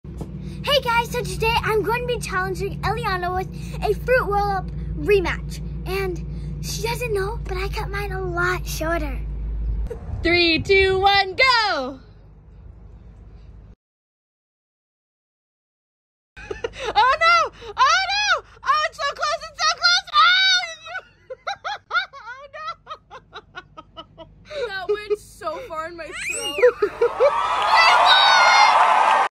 Fruit Roll-Up Challenge 🍓
Time for an EPIC Youtube short Fruit Roll-Up Challenge rematch with Elliana Walmsley. This trending challenge will be a rematch is one for the history books. Do you I think can finally defeat the 100 times world champion or return home disgraced and defeated. This insanely fun food challenge can also be seen on TikTok. Do you like this Food Challenge? Should I do more?? What Famous Youtuber should I Challenge Next?